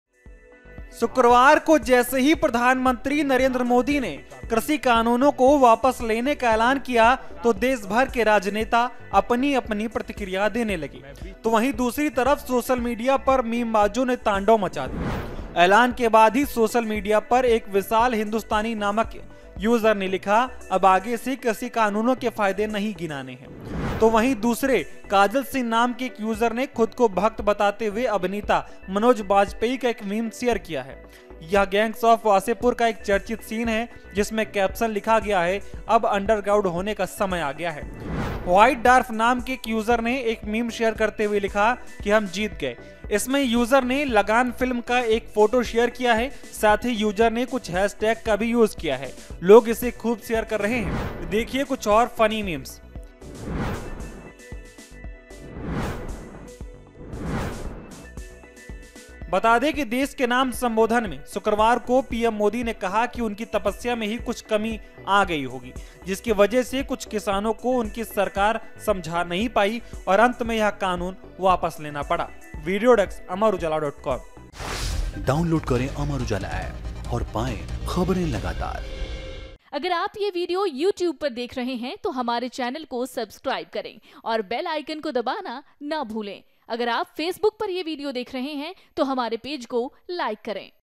Social Media Memes on Withdrawal of Farm Laws | कृषि कानून वापस होने पर मीमबाजों ने मचाया तहलका
कृषि कानून वापस होने पर Social Media पर मीमबाजों ने घमासान मच दिया है और तमाम फनी रिएक्शन देखने को मिल रहे हैं।कुछ लोग तो वापसी के ऐलान का स्वागत कर रहे हैं तो वहीं कुछ लोग इससे नाराज भी नजर आए।